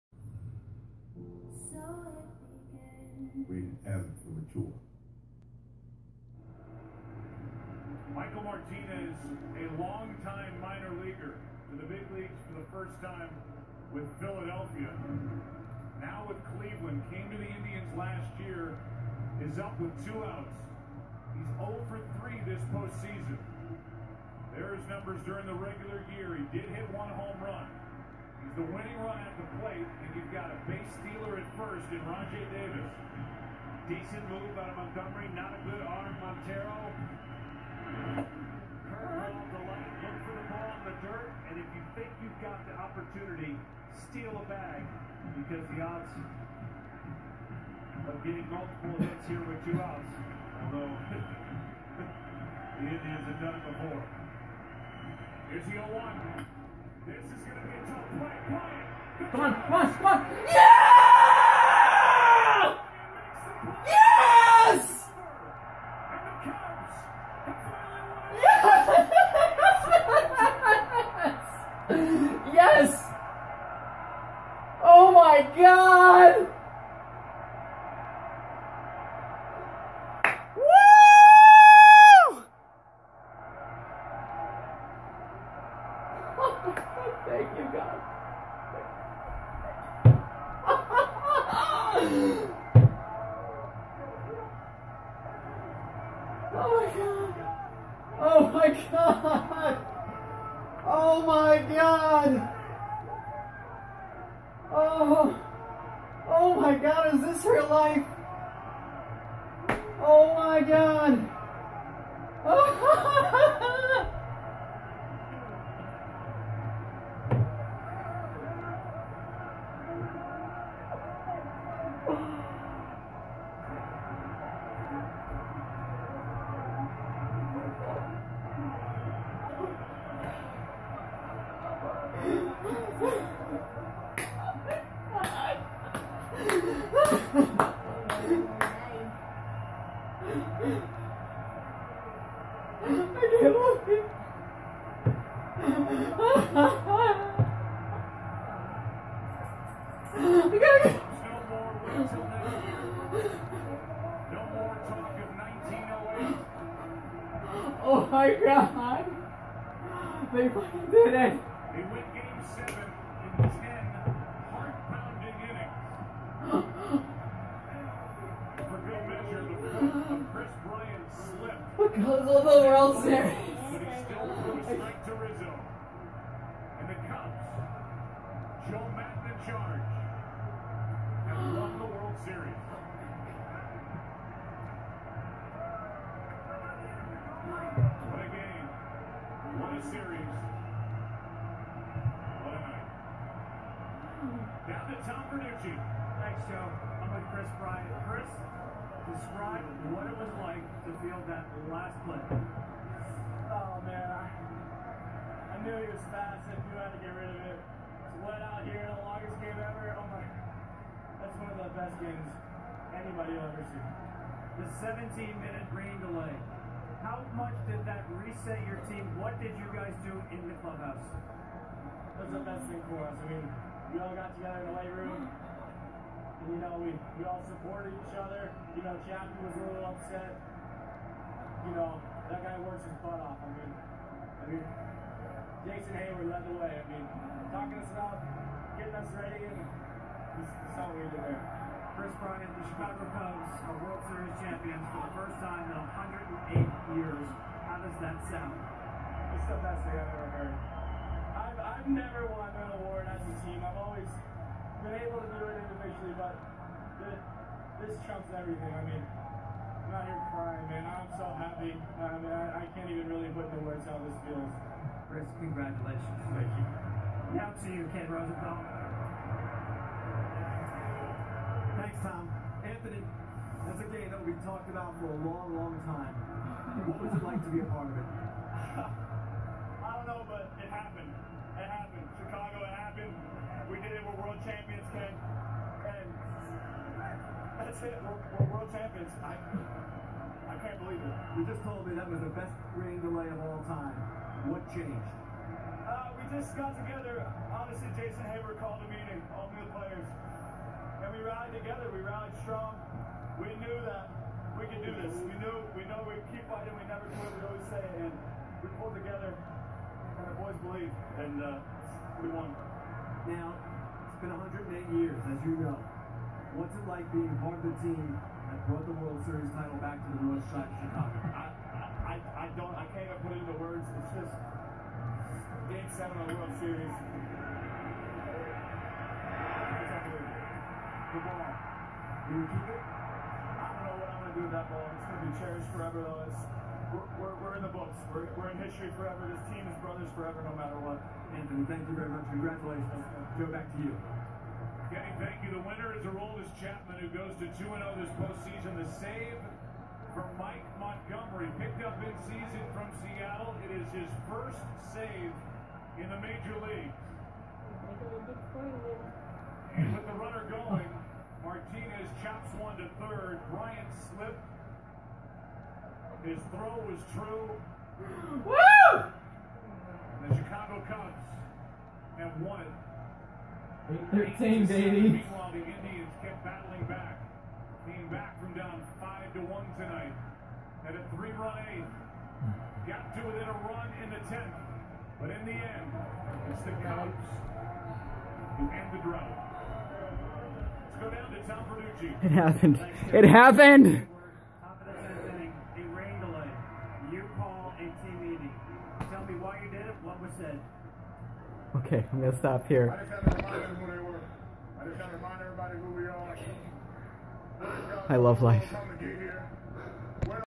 My reaction to the Cubs winning the World Series
I have been a life long fan of the Chicago Cubs.  Pretty much the best day of my life!  The Chicago Cubs are finally World Series Champions!!!